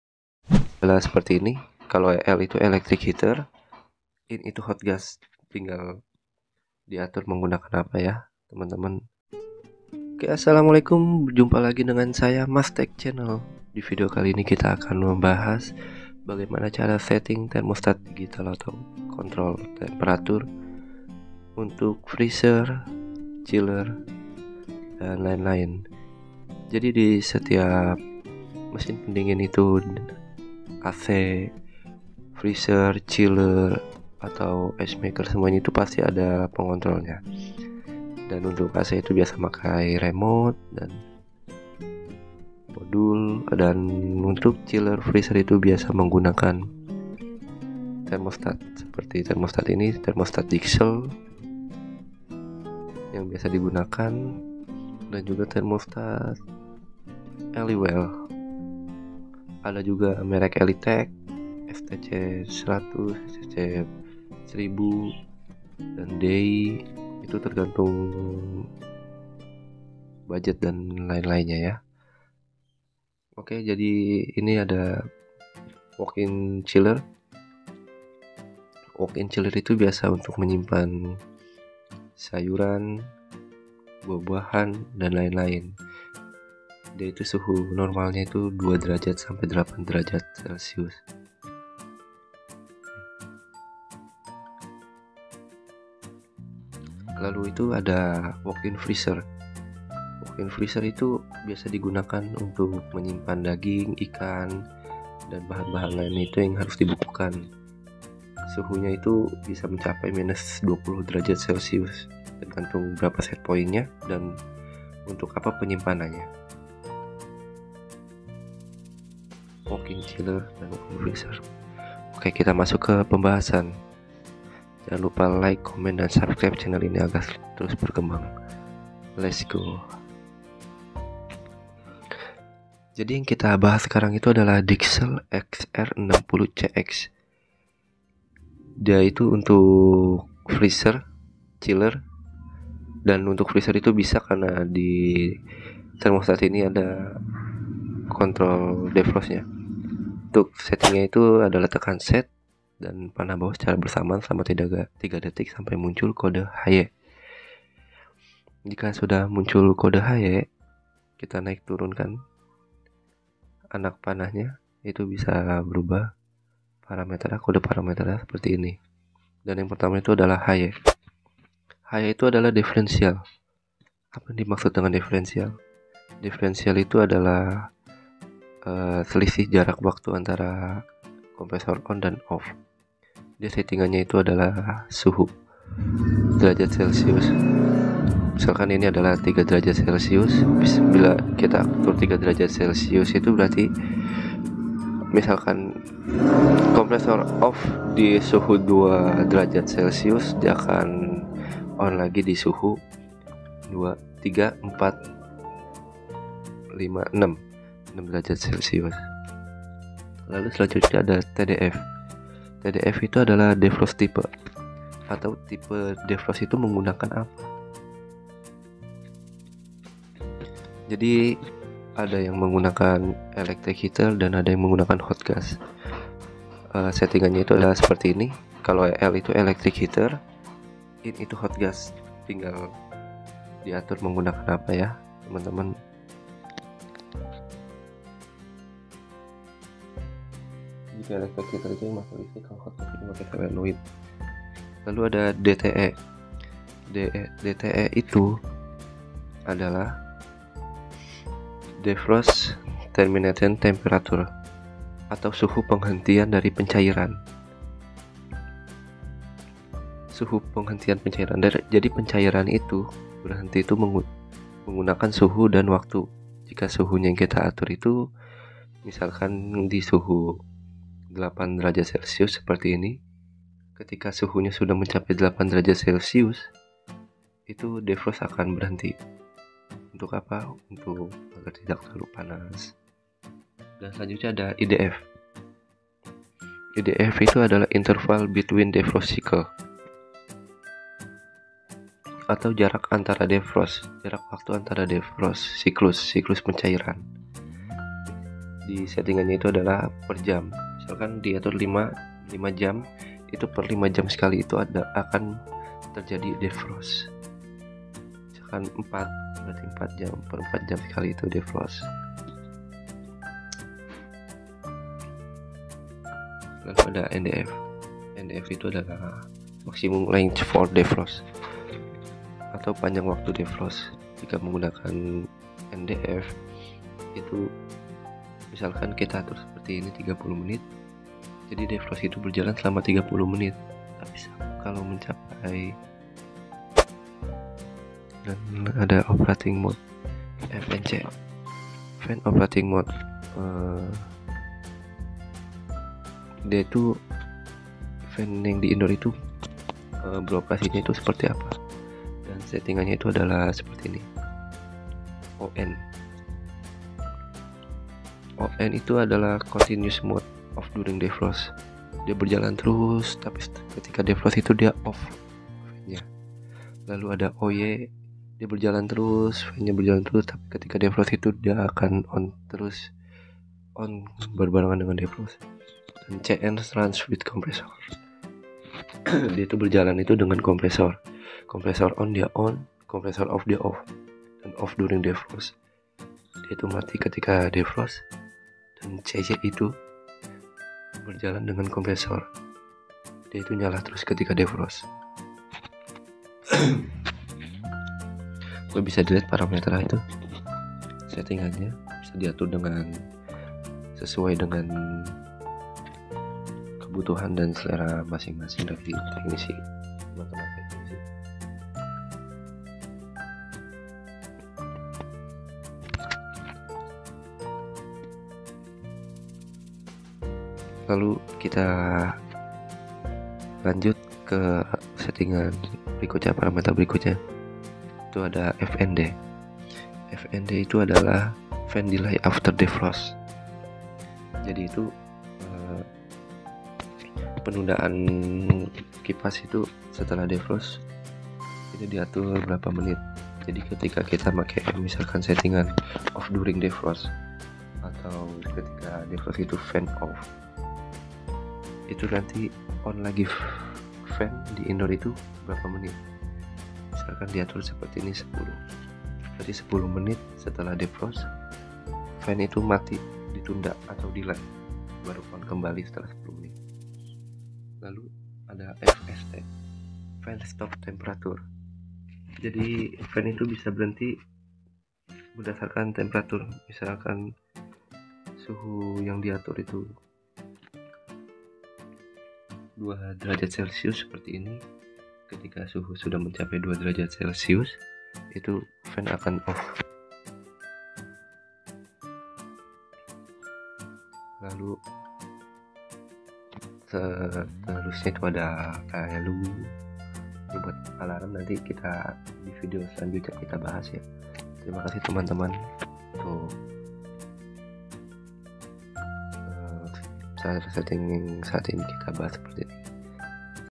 cara setting Dixell (arti kode parameter)Dixell XR60CX
Berjumpa lagi dengan MAStech channel,di video kali ini saya akan membahas bagaimana cara setting temperatur control dixell XR60CX,dan juga akan diberitahu arti dari kode parameter seperti:
Hy,tdf,dte,idf,ndf,Fnc,fst

divideo kalo ini akan membahas arti parameter untuk defrost dan pengaturan fan indoor unit